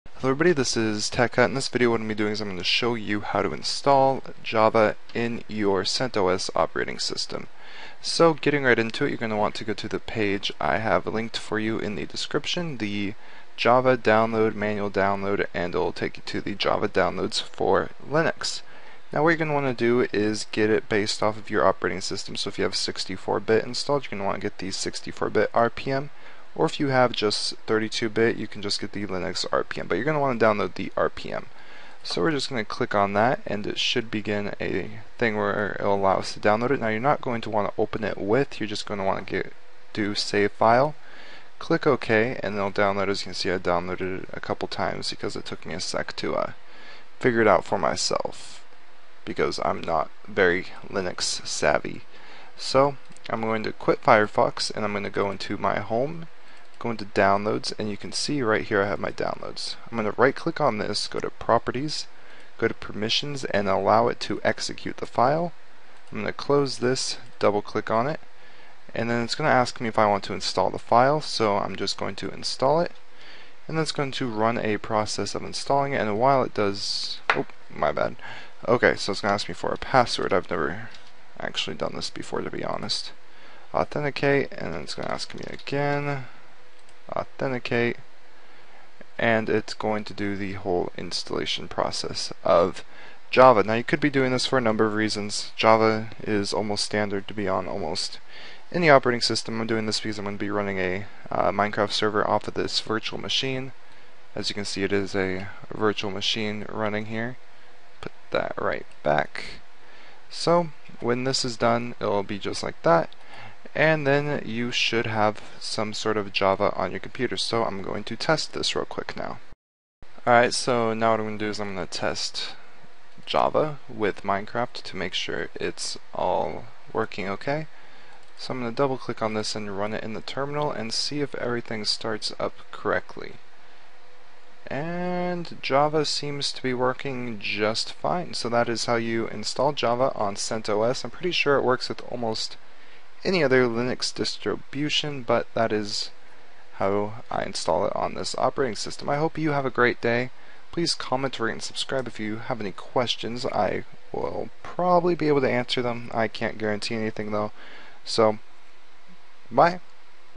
How to install Java in CentOS and other Linux Distros
In this quick tutorial I will be showing you how to install java in CentOS, this method will also work for other linux distros such as Ubuntu, Debian, and Linux Mint.

Link Download in a new tab)